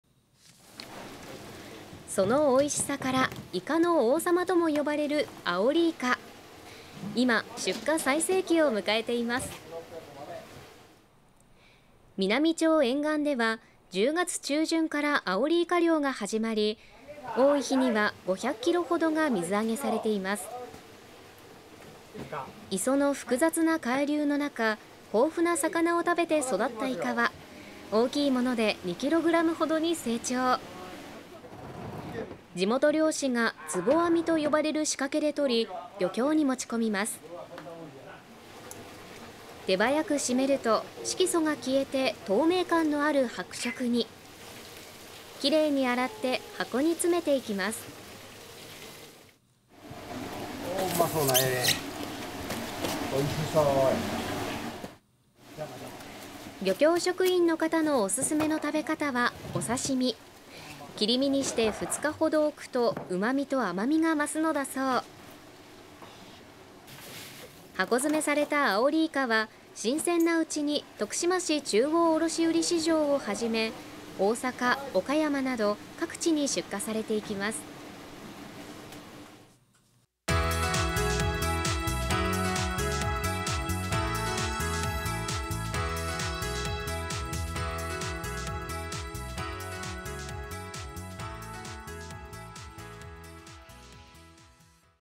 県南の特産 アオリイカ出荷最盛期【テレビトクシマ】
県南・美波町でアオリイカ漁が最盛期を迎え、漁港では
出荷作業に追われています。